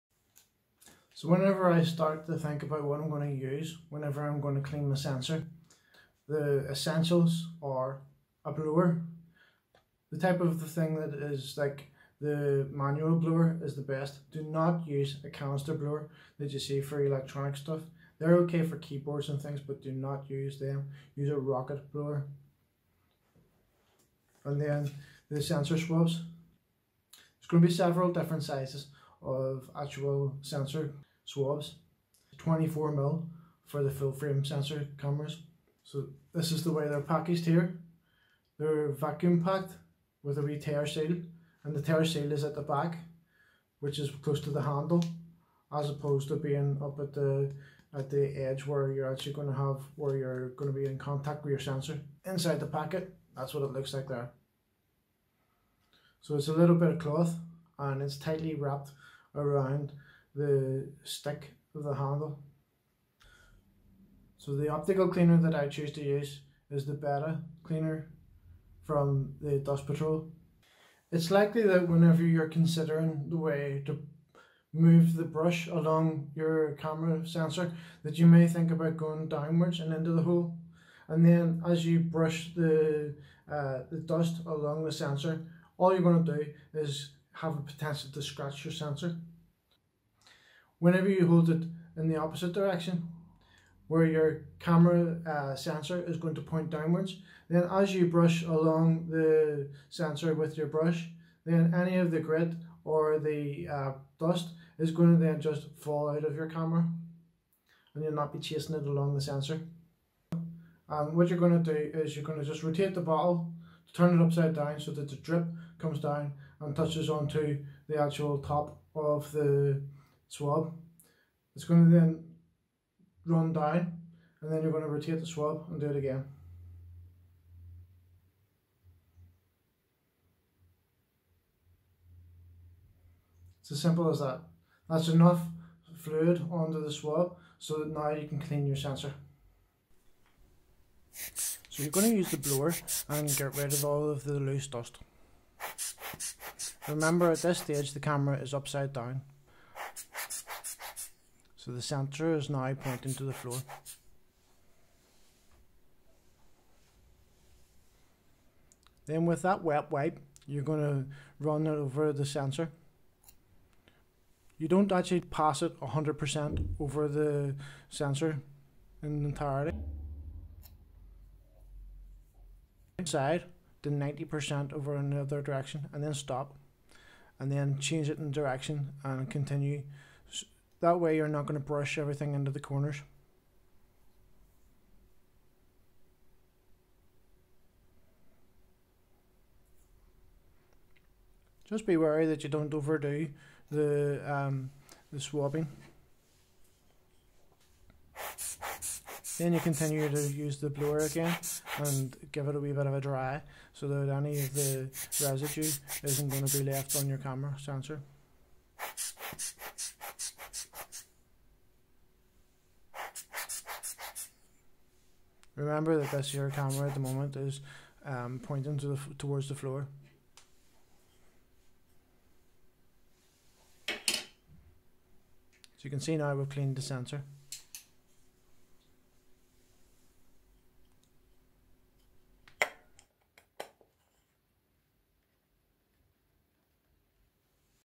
How to clean your Mirrorless Camera Sensor - S Black Photo
Clean your sensor on the mirrorless camera explained in a how to video.
I discuss how I clean the sensor on my Sony a7iii

The supplies I used.
dust patrol fluid and sensor swabs
Sensor Swabs
Small Rig for Sony a7iii
Sony a7iii

Bringing you the Photographs of Steven Black from S Black Photo.
Images of Ireland Landscapes and nature birds, animals wild or captive.